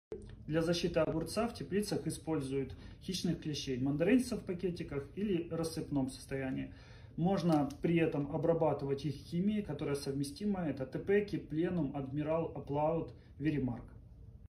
Чем обрабатывать огурец от Белокрылки?
Хищные клещи, энтомофаги хорошо контролируют Белокрылку и останавливают резистентность к химии.